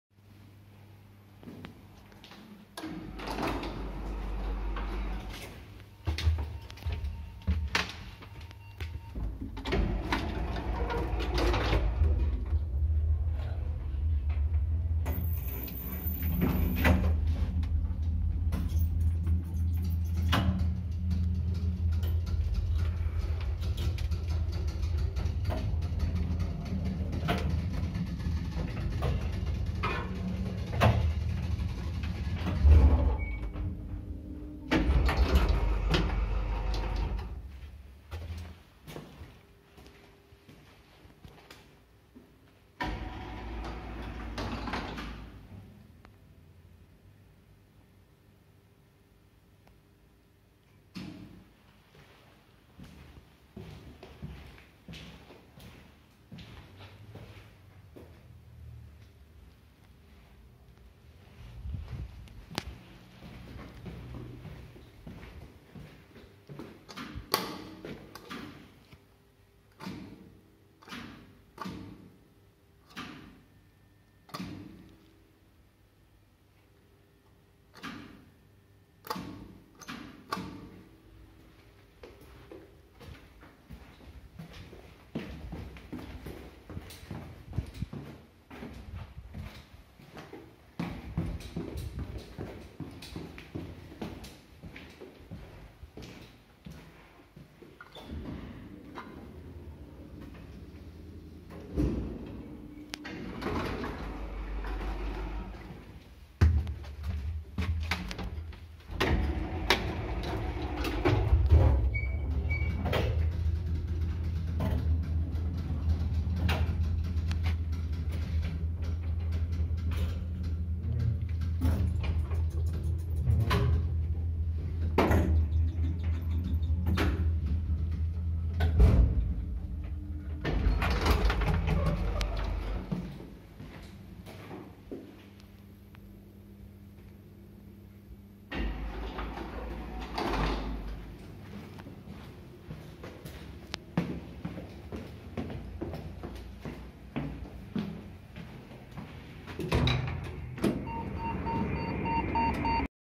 (Неисправный этажный переключатель) Лифт КМЗ, раб. с 1987 г., V=0,71 м/с, г/п 320 кг
МО, Город Электросталь, Улица Юбилейная, дом 1А, подъезд 4.